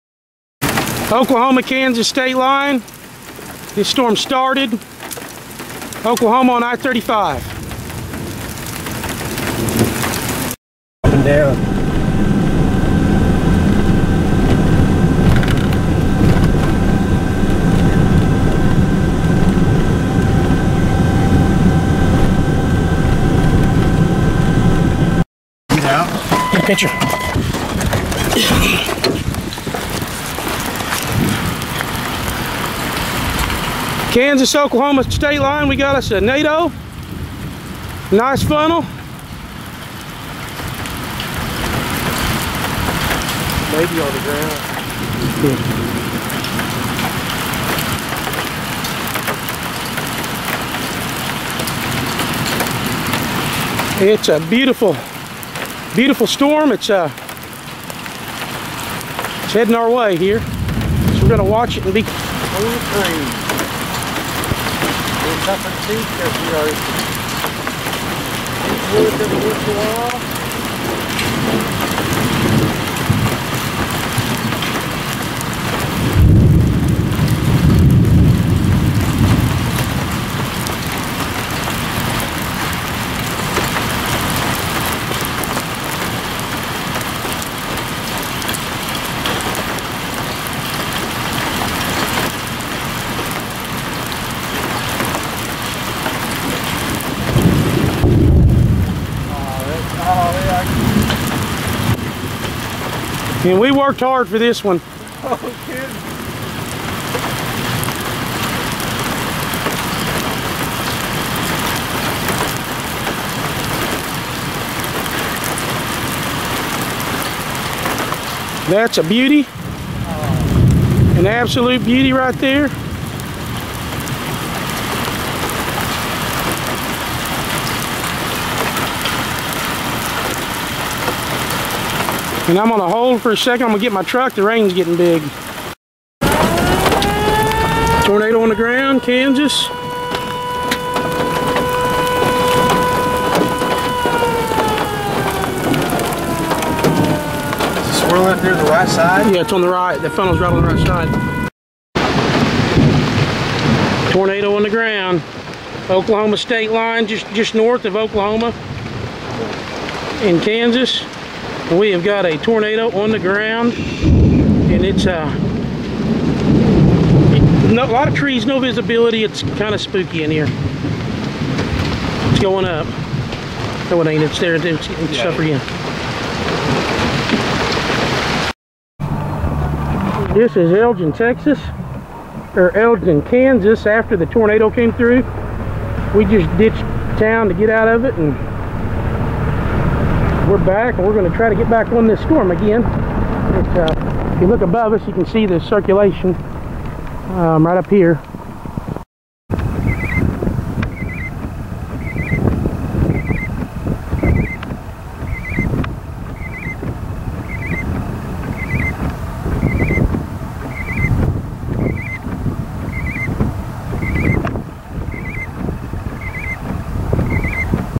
OKLAHOMA & KANSAS TORNADO CHASE June 18th 2011
TORNADO CHASING KANSAS OKLAHOMA STATE LINE
FROM ARKANSAS CITY TO ELGIN KANSAS
WE FOUND A NICE ROPE TORNADO.